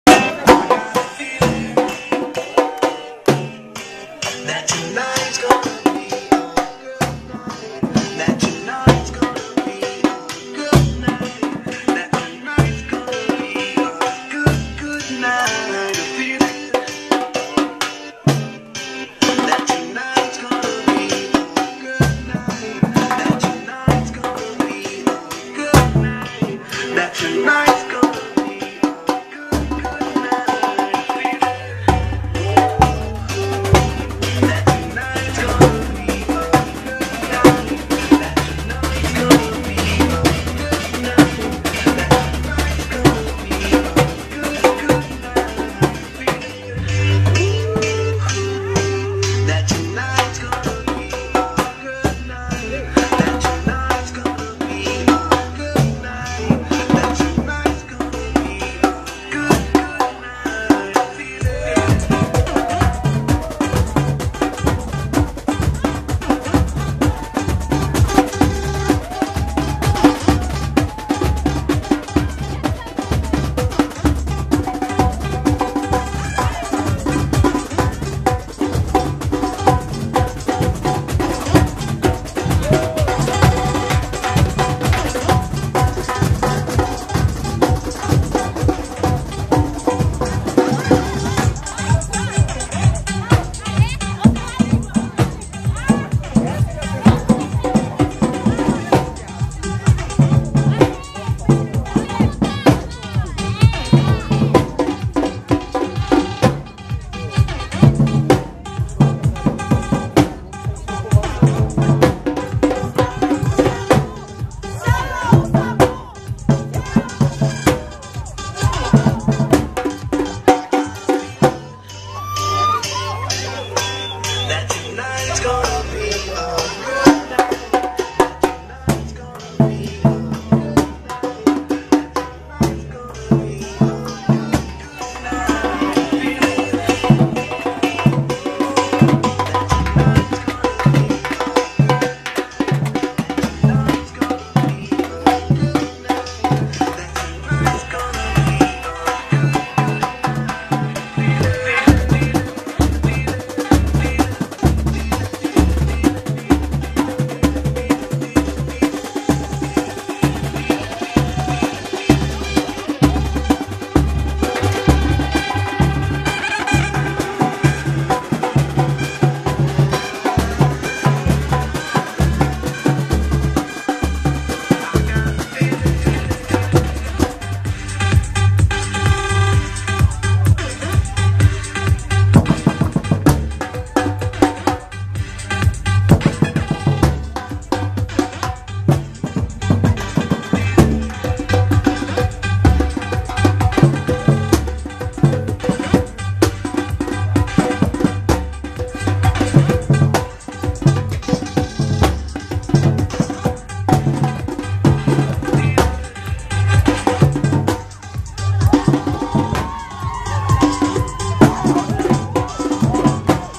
TechnoFervo - Mistura de música eletrônica com bateria de escola de samba - Fervo Samba Show
A mistura que dá o que falar.
O melhor show de música eletrônica, com um DJ incrível da noite paulistana, misturado com a melhor bateria show do Brasil, o Fervo Samba Show.
Essa mistura resulta no TechnoFervo, que há mais de 10 anos dominam o cenário nessa modalidade com apresentações por todo o Brasil e apresentações internacionais.
Leve já esse show para o seu evento.

Contate-nos